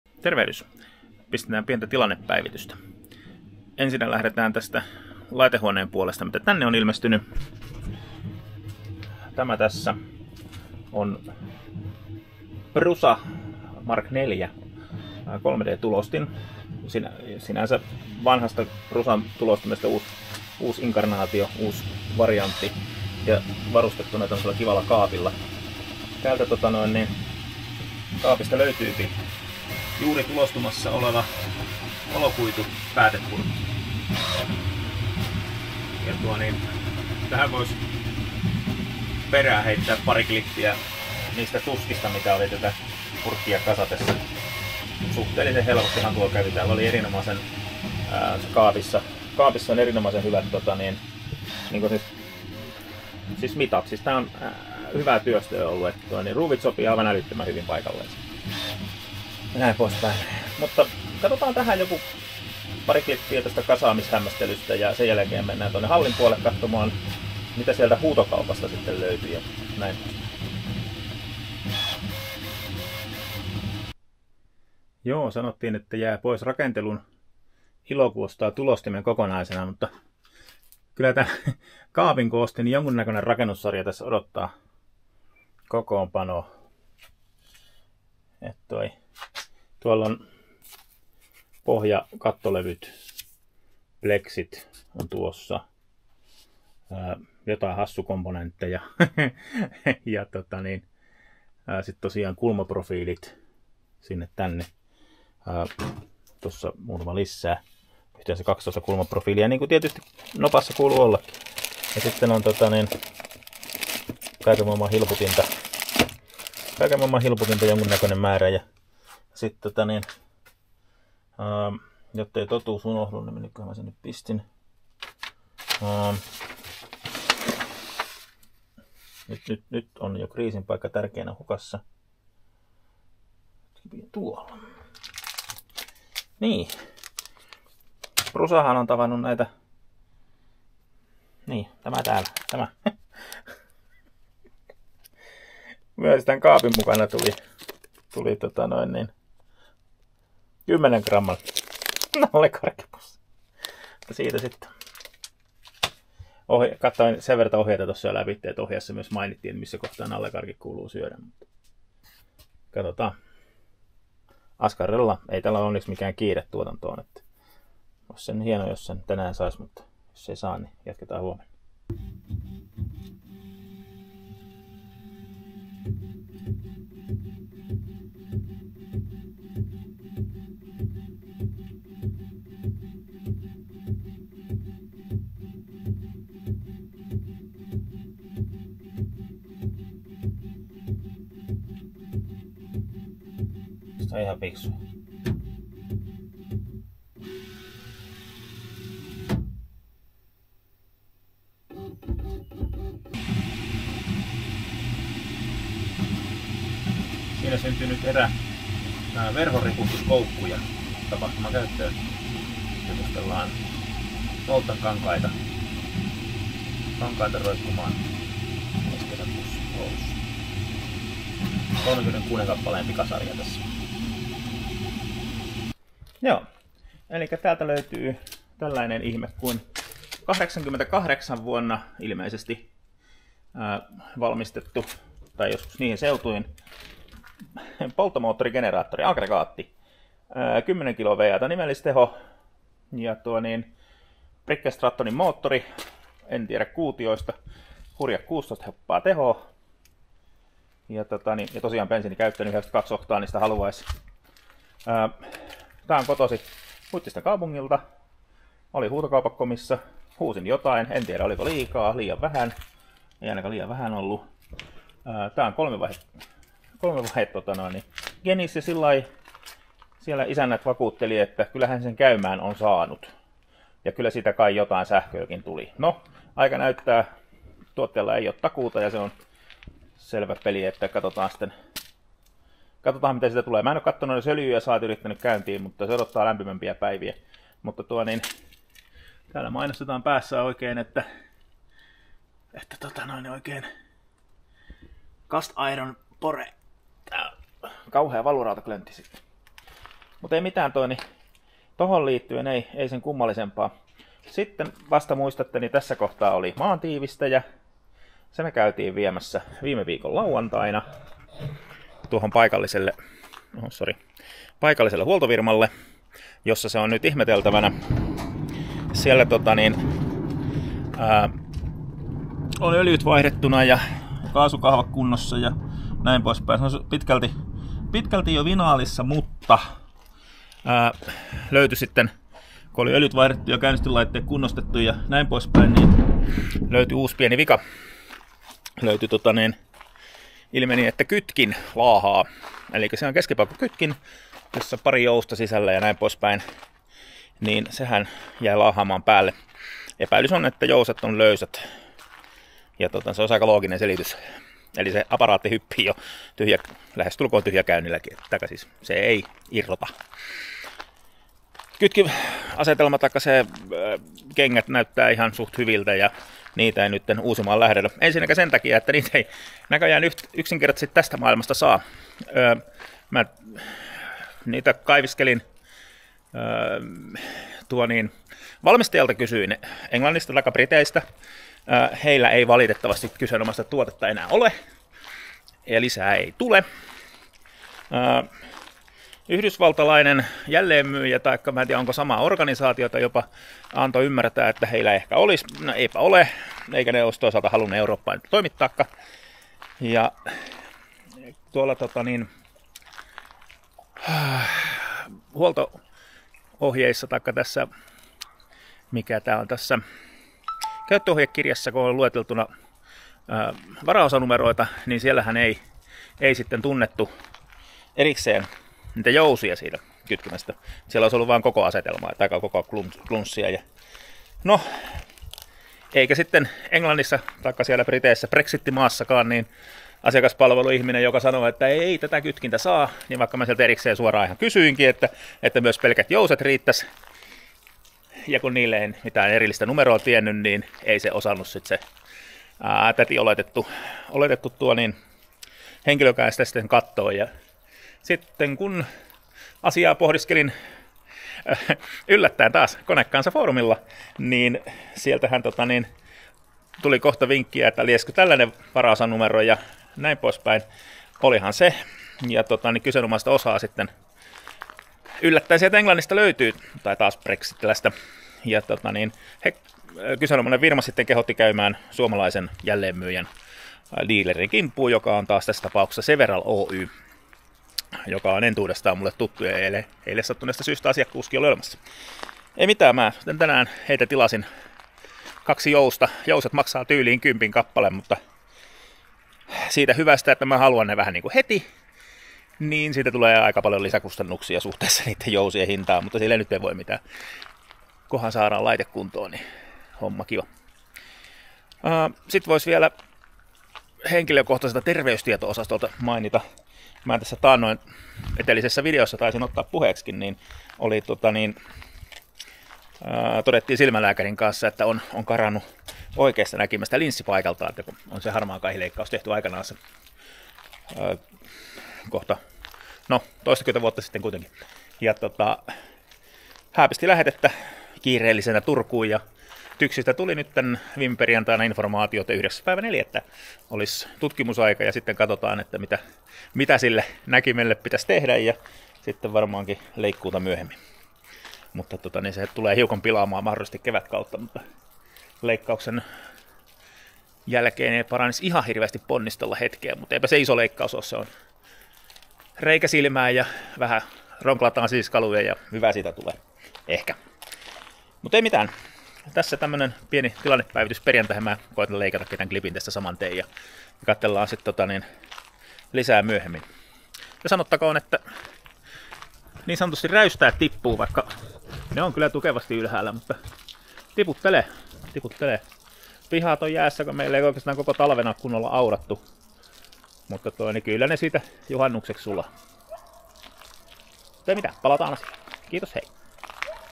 Kalustohankintoja ja silmäänsahausta - Perjantain tilannepäivitys
Pari viikkoa on jälleen vierähtänyt ja on aika päivittää kuulumisia.
On tullut huudettua kaupassa, ostettua ihan uuttakin lelua ja tuli aika sinne tutkimuksiinkin.